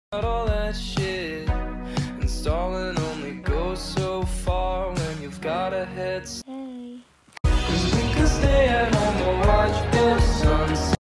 “Hi” [] meme [] Voice Reveal! [] tysm for 500!
I wanted to do a voice reveal since I have almost 600 and I will do a age and maybe a face reveal when I get to 1k